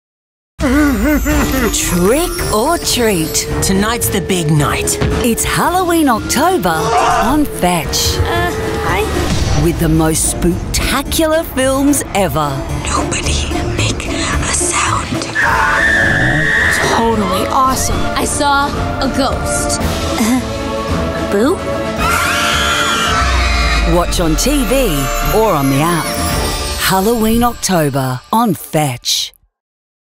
October - Kids Halloween
It’s Halloween October on Fetch.

With the most spooktacular films ever.

Watch on TV or on the app.

Halloween October on Fetch.